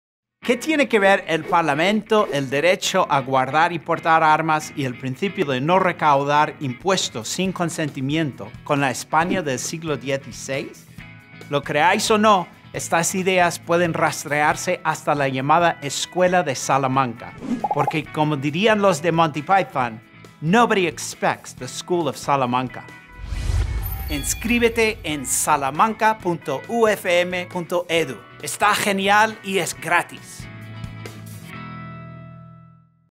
Curso en línea La Escuela de Salamanca - salamanca.ufm.edu
Inscríbete al curso en línea: salamanca.ufm.edu

La Escuela de Salamanca es un curso MOOC de la Universidad Francisco Marroquín que consta de cinco capítulos. La Escuela de Salamanca es un término usado para describir un amplio grupo de intelectuales de los siglos dieciséis y diecisiete de España, Portugal y Latinoamérica. El curso en línea consiste de cinco capítulos: una introducción en homenaje a su fundador Francisco de Vitoria; un capítulo de la contribución de la Escuela de Salamanca sobre derechos humanos, política y economía; y una conclusión en homenaje al trabajo de Juan de Mariana. Aprende sobre los orígenes del liberalismo hispano, así como su influencia en la civilización occidental moderna. Gana las insignias al completar las actividades del curso.

Eric Clifford Graf es catedrático de literatura en la Universidad Francisco Marroquín. Se doctoró en literatura española en la Universidad de Virginia (1997), sus áreas de especialización incluyen: literatura española medieval y moderna, filosofía renacentista, historia de la novela y teoría literaria, política, cultural y económica.

Créditos:
New Media | UFM 2017
salamanca.ufm.edu
newmedia.ufm.edu
ufm.edu